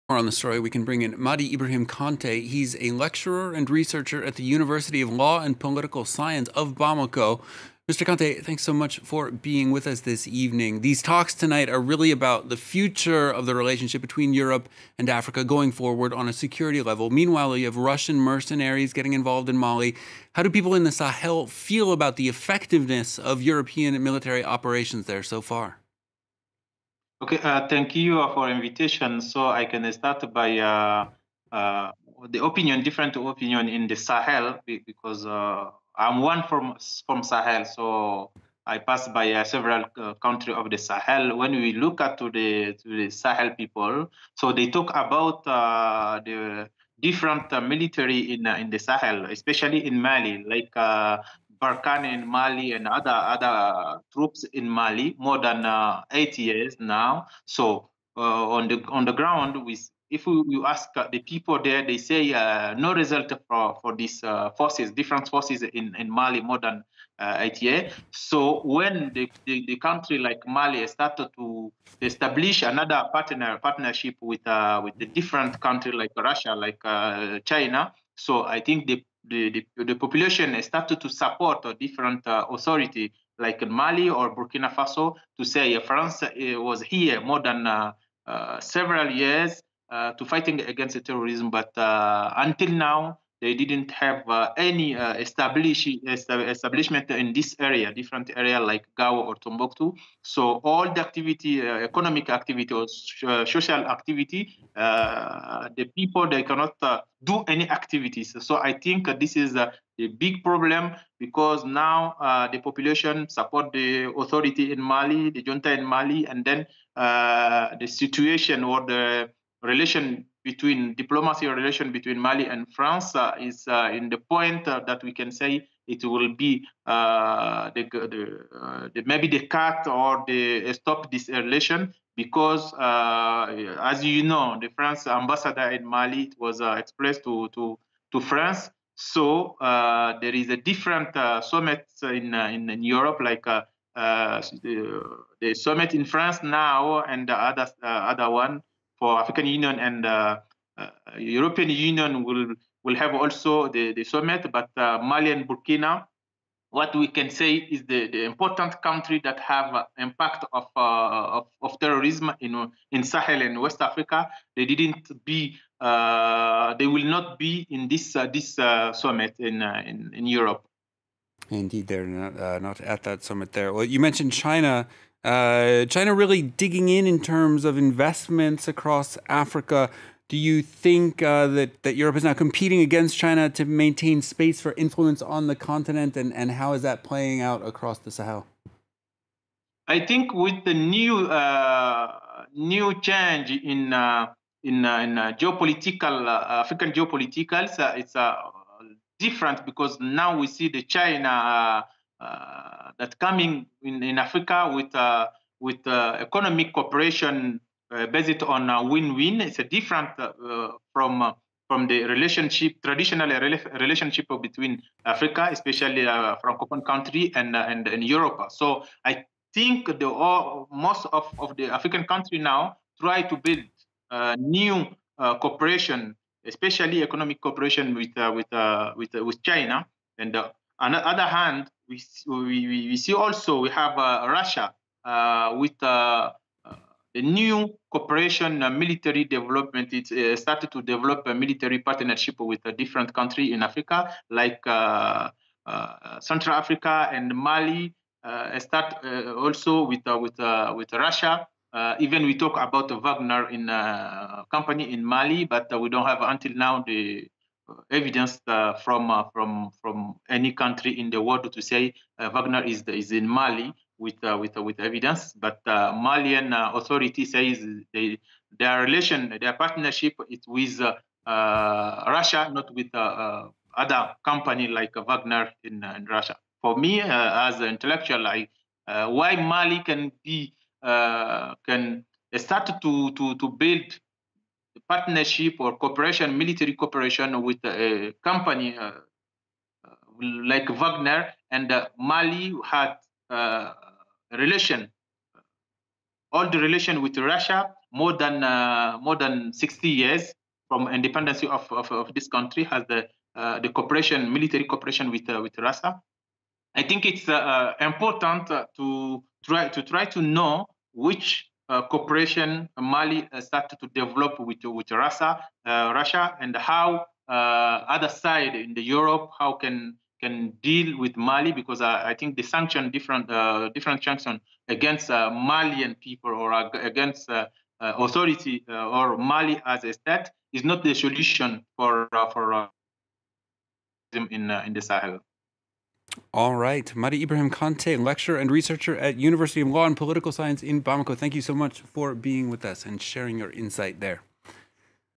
France 24 English - Mady I. Kanté, the future of the military cooperation between Europe and Africa
France 24 English - Mady Ibrahim Kanté, lecturer at the University of Law and Political Science of Bamako, talks about the future of the military cooperation between Europe and Africa